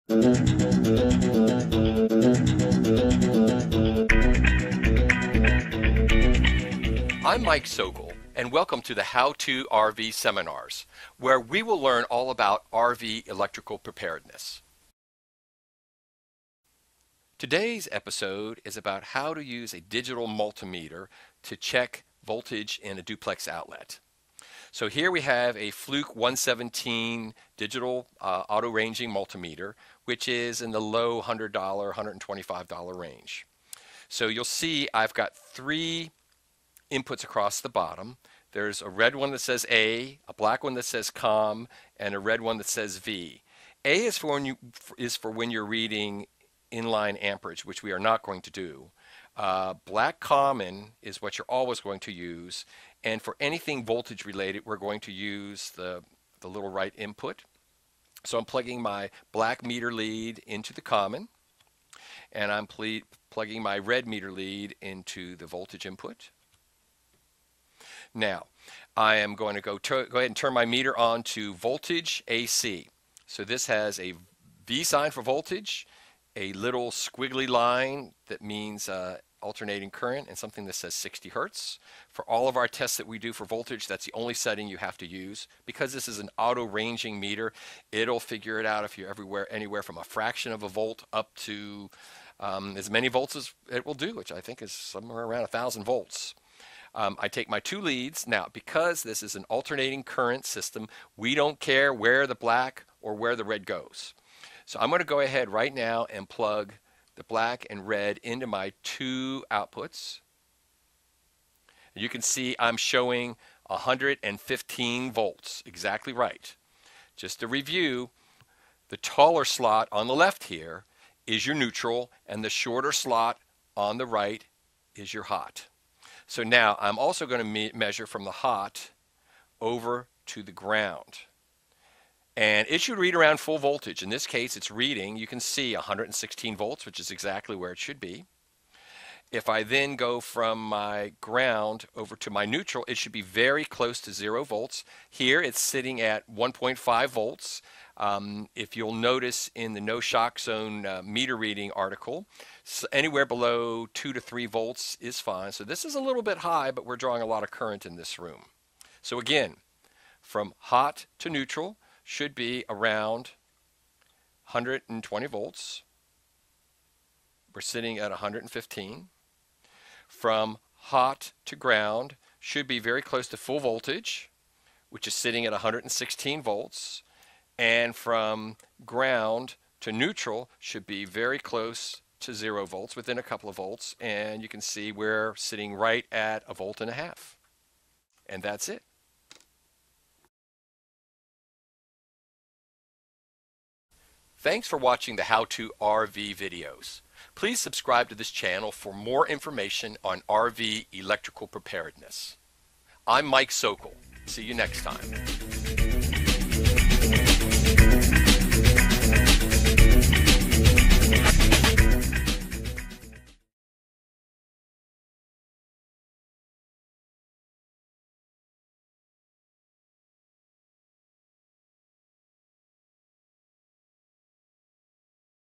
How-To use a digital multi-meter
Using a digital multi-meter to read the Hot, Neutral, and Ground voltage levels on a duplex outlet. for more articles about electrical safety.